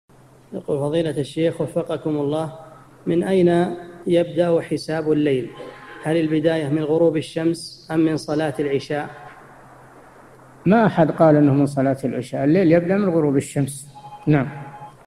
[1274 /1350] من أين يبدأ حساب الليل؟ من غروب الشمس، أم من صلاة العشاء؟ الشيخ صالح الفوزان
-[1274 /1350] السؤال: من أين يبدأ حساب الليل؟ هل البداية من غروب الشمس، أم من صلاة العشاء؟
الجواب: ما أحد قال أنه من صلاة العشاء، الليل يبدأ من غروب الشمس.
للاشتراك في قناة الشيخ صالح الفوزان
أسئلة وأجوبة من شرح قاعدة جليلة ( 1350 ) مقطع

للاستفسار والإقتراحات والدعم
رئيس الفريق
لمشروع كبار العلماء